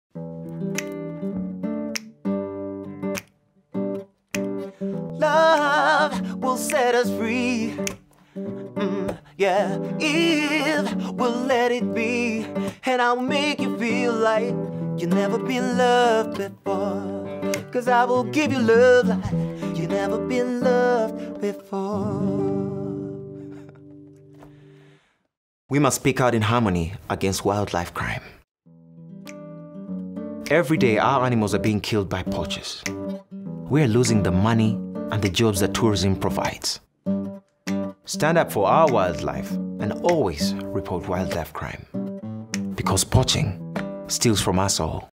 Maurice Kirya "Poaching Steals From Us All" Uganda En
The Uganda Wildlife Authority, Uganda Conservation Foundation and WildAid today launched the “Poaching Steals from Us All” public awareness campaign featuring musician and actor Maurice Kirya.